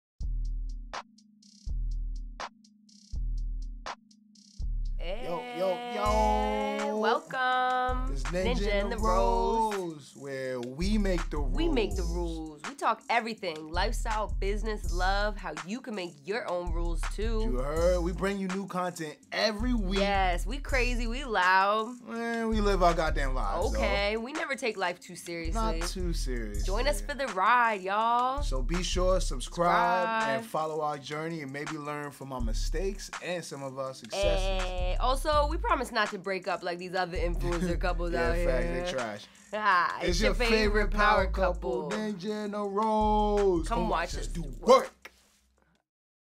Interracial Power Couple Talks Love, Business, Relationships And Goals
Welcome to Nenjah & The Rose!

Come laugh and grow with us on our self made journey through life! New content every week!